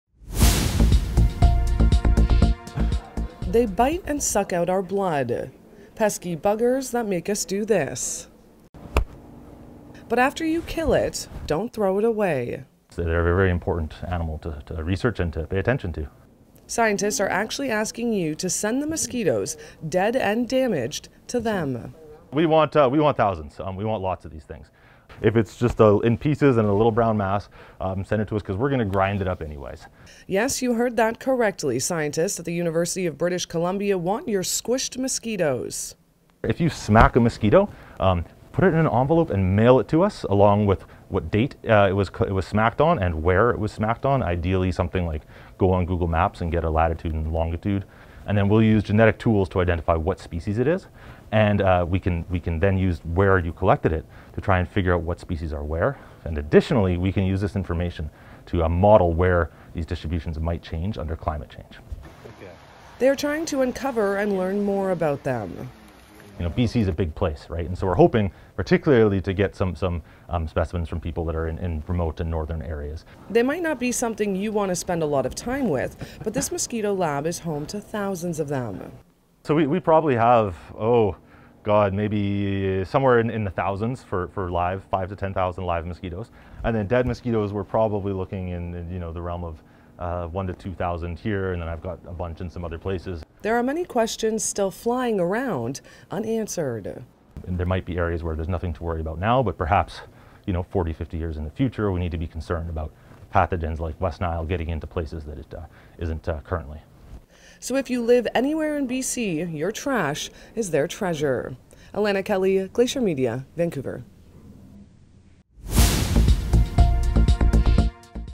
B.C. scientist wants your dead mosquitoes
As climate change expands the northward range of many species, a new citizen science project is calling on B.C. and Yukon residents to send in slapped mosquitoes so researchers can track them and the diseases they may carry.